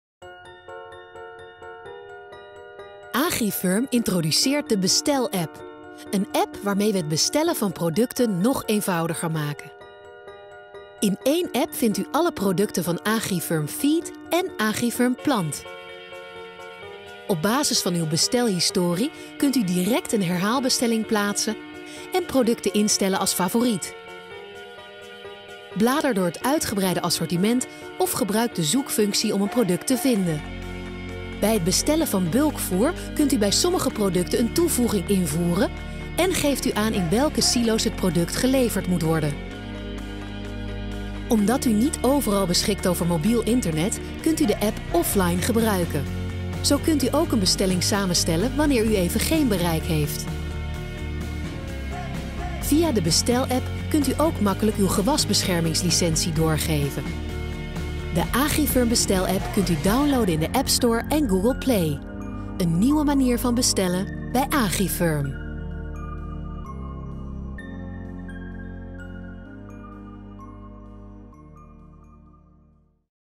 Mobiel bestellen met de Agrifirm BestelApp
Of u nu onderweg, offline, in de schuur of op het land bent. Met de app kunt u altijd en overal bestellen. Verstuur uw bestelling direct of verzamel deze tijdens de dag in uw ‘boodschappenlijstje’. U geeft aan wanneer u de producten geleverd wilt krijgen en ontvangt direct terugkoppeling zodra de bestelling is ontvangen en verwerkt.